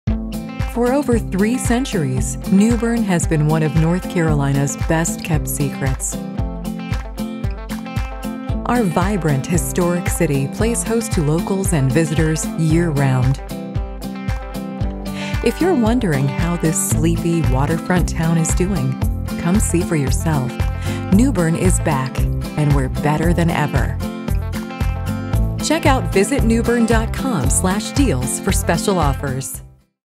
New Bern, NC | We're back (Deals)
New Bern is the first state capital of North Carolina, home to Tryon Palace and the birthplace of Pepsi Cola. Discover the NC History Center, enjoy drinks and dinner on the waterfront, or shop at one of our unique downtown boutiques. Discover all the NEW things happening in this charming city that's over 300 years old.

for more information.